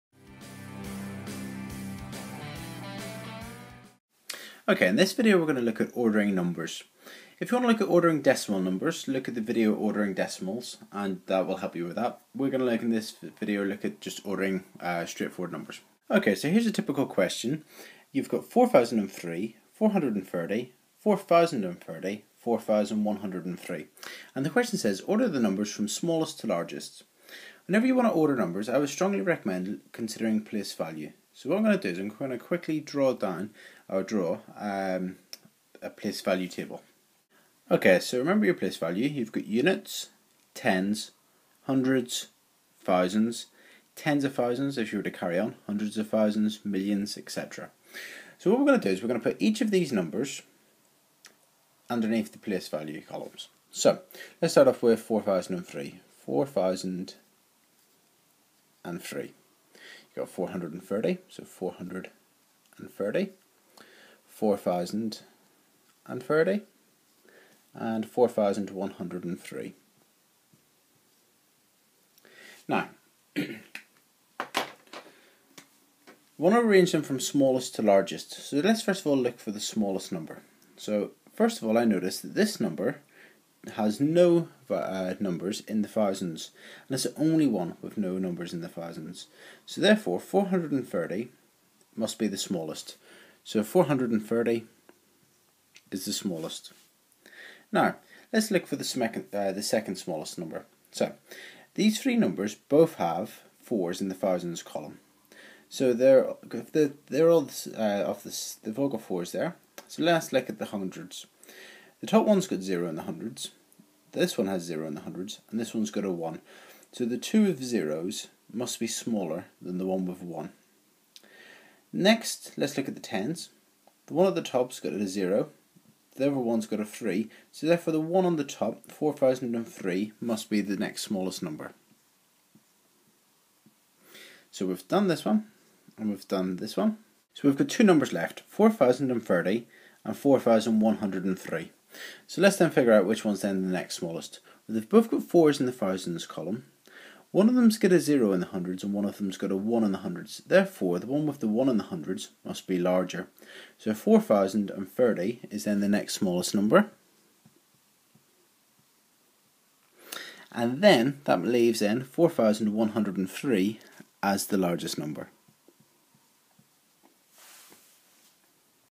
Ordering Numbers - Corbettmaths
This video explains how to order numbers.